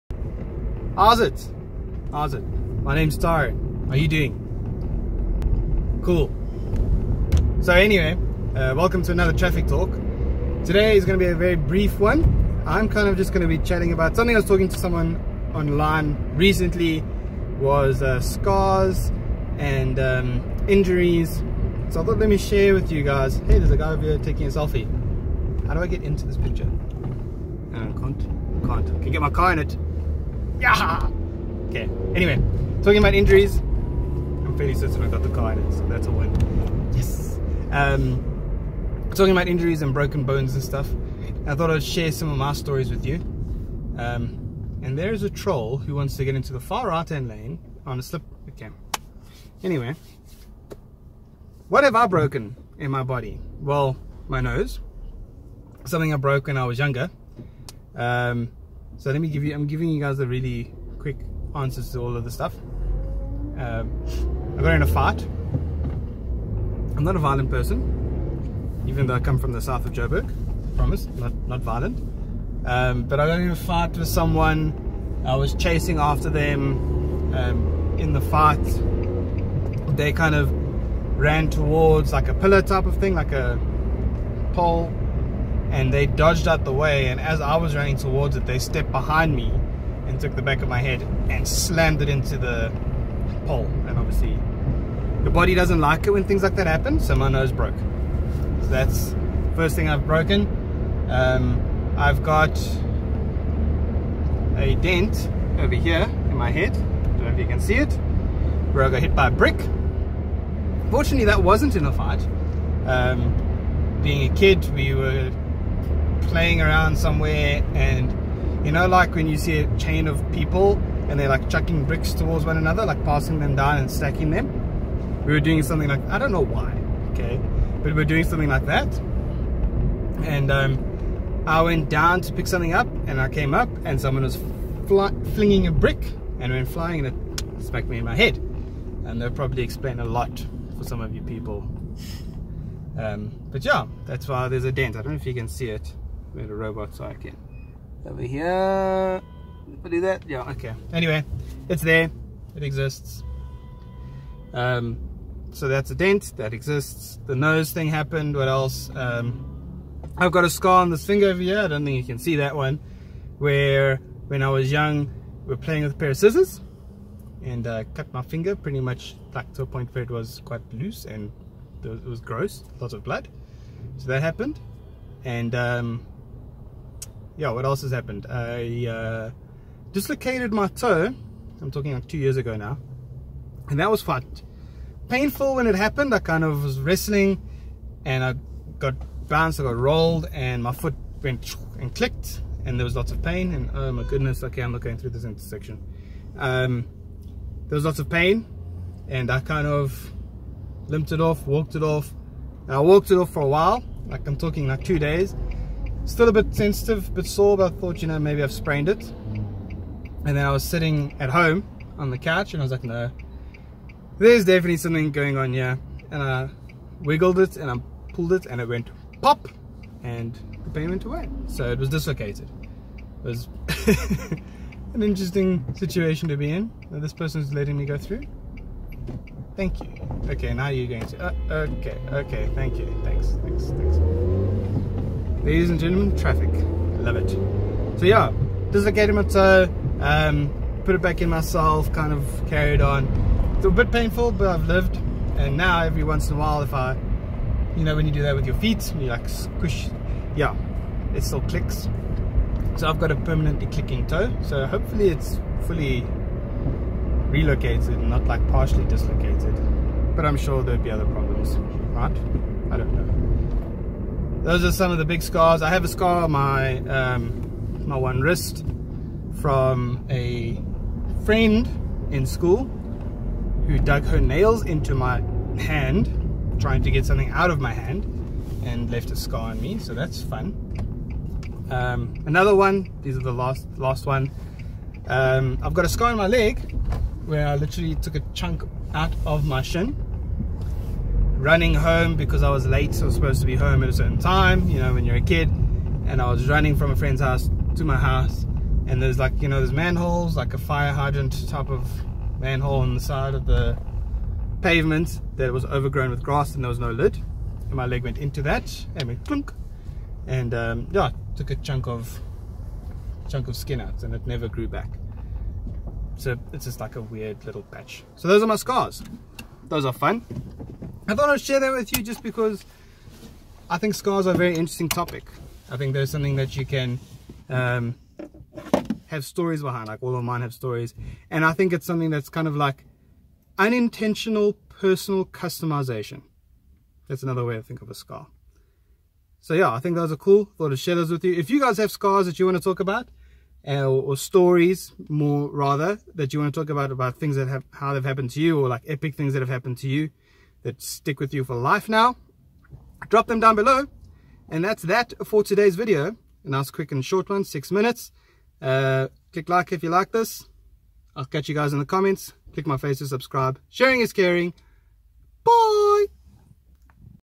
Traffic Talk S2E9: Scars
Timestamps below. Catch you in the comment :)

0:00 Howzit
0:22 I photobombed him
0:50 My various injuries
5:54 K.TX.Bye